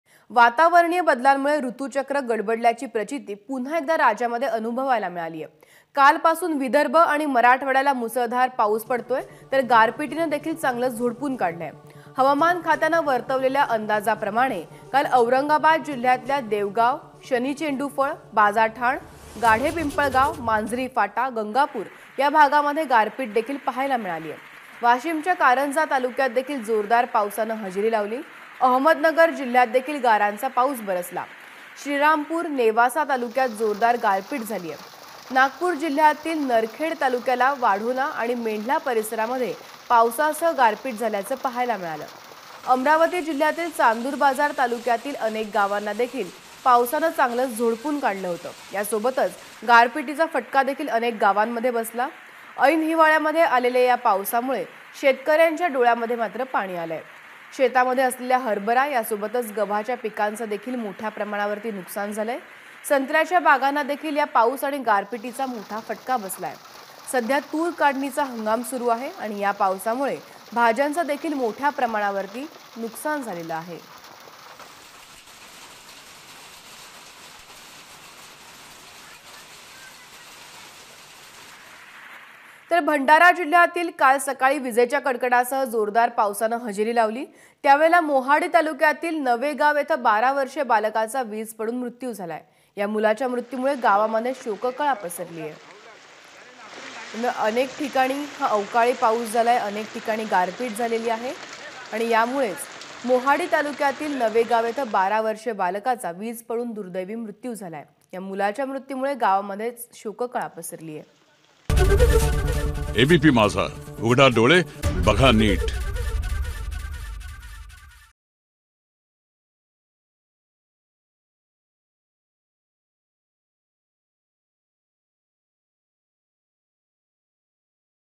Maharashtra Hailstorm : राज्यातील 11 जिल्ह्यांना अवकाळी पावसाने झोडपलं ABP Majha
वातावरणीय बदलांमुळे ऋतुचक्र गडबडल्याची प्रचिती पुन्हा एकदा राज्यात अनुभवायला मिळतेय. कालपासून विदर्भ आणि मराठवाड्याला मुसळधार पाऊस आणि गारपीटीनं झोडपलंय. हवामान खात्यानं वर्तवलेल्या अंदाजाप्रमाणे काल औरंगाबाद जिल्ह्यातल्या गंगापूर, वैजापूर तालुक्यात जोरदार काल पाऊस झालाय. देवगाव, शनि चेंडूफळ, बाजाठाण, गाढेपिंपळगाव, मांजरी फाटा, गंगापुर या भागात गारपीट ही पाहायला मिळाली.. वाशिमच्या कारंजा तालुक्यातही जोरदार पावसाने हजेरी लावली. सध्या तूर काढणीचा हंगाम सुरू आहे..तर हरभरा, फळभाजी पिकांचं  नुकसान झालंय.अहमदनगर जिल्ह्यातही गारांचा पाऊस झाला.. श्रीरामपूर, नेवासा तालुक्यात जोरदार गारपीट झाली. अचानक झालेल्या पावसाने नागरिकांसह बळीराजाची चांगलीच तारांबळ उडाली.हवामान विभागाने विदर्भात २ दिवस ऑरेंज अलर्ट दिलाय.. त्यानुसार)) नागपूर जिल्ह्यातील नरखेड तालुक्यातल्या वाढोना आणि मेंढला परिसरात पावसासह गारपीट झालीय. यामुळे संत्रा पिकाचं नुकसान झालंय.अमरावती जिल्ह्यातील चांदूरबाजार तालुक्यातील अनेक गावांना पावसासह गारपिटीचा फटका बसला.  ऐन हिवाळ्यात आलेल्या या पावसामुळे शेतकऱ्यांच्या डोळ्यात मात्र पाणी आलेय. शेतात असलेल्या हरभरा तसेच गव्हाच्या पीकांच नुकसान झालं आहे. संत्र्याच्या बागांनाही या पाऊस आणि गारपिटीचा फटका बसलाय. सध्या तूर काढणीचा हंगाम आहे.. या पावसामुळे भाज्यांचंही मोठं नुकसान झालंय.